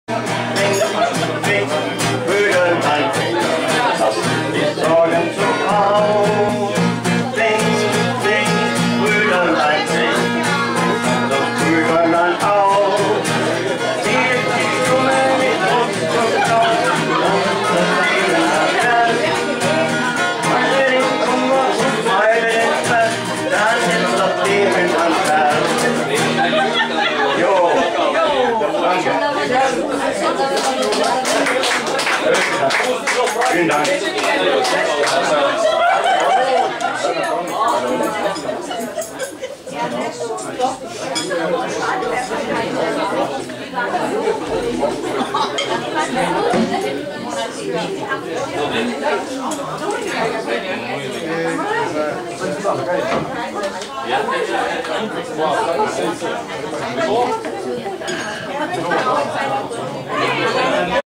Michael Rostalski Musiker - Gitarre & Gesang
Geburtstage - Firmenfeiern

Die Musik lädt ein zum mitmachen, schunkeln und tanzen! Musik die man kennt zum mitsingen!